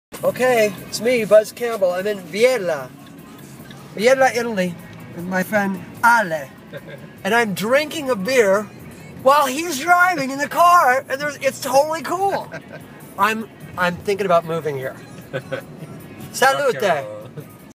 Beer in the car in Biella, Italy.
Buzz discusses drinking a beer in the font seat of a car in Biella, Italy.